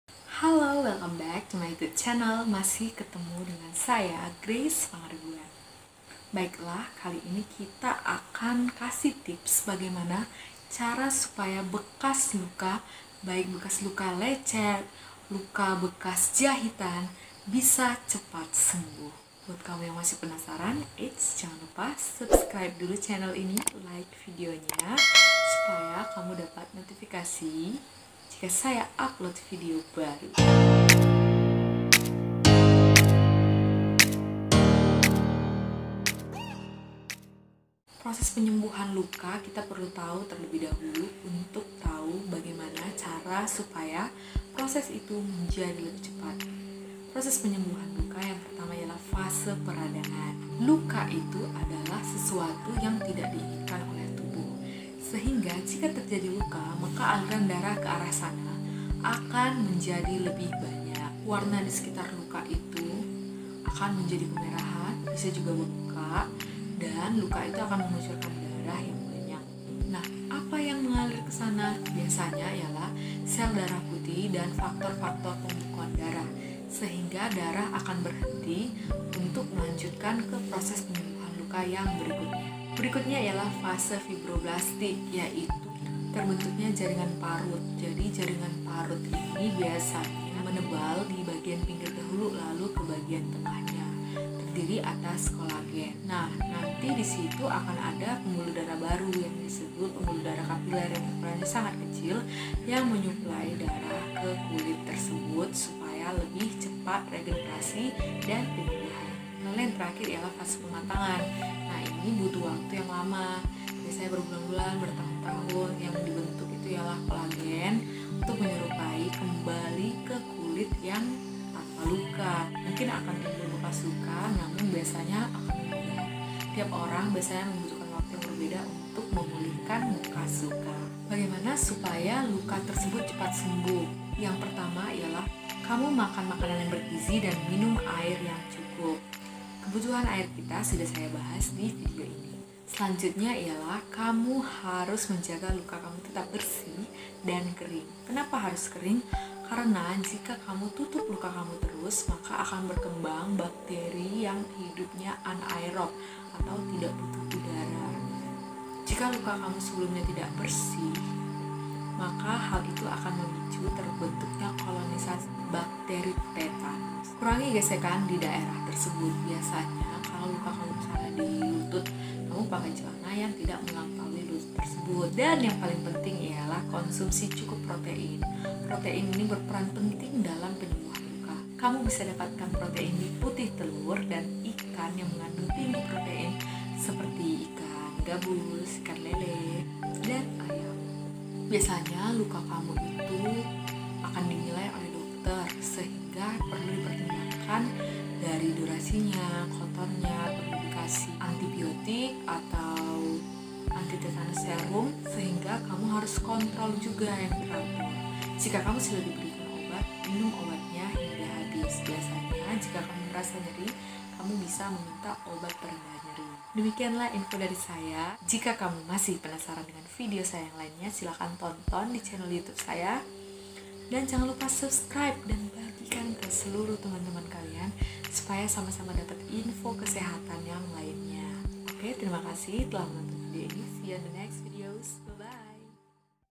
Alternatif Menghilangkan Bekas Luka Tanpa Laser
Bekas luka memang menyebalkan. Selain mengganggu penampilan, juga bisa menurunkan rasa percaya diri alias tidak PeDe. Namun ternyata, bahan-bahan alami ini bisa menghilangkan bekas luka, loh.
Saya sendiri sudah coba dan berhasil!
Yuk, simak videonya sampai habis!

Halo semua, tanyadoktergrace telah hadir di Grup Telegram sebagai wadah diskusi, konsultasi, dan berbagi...
Silakan klik link berikut untuk bergabung ya...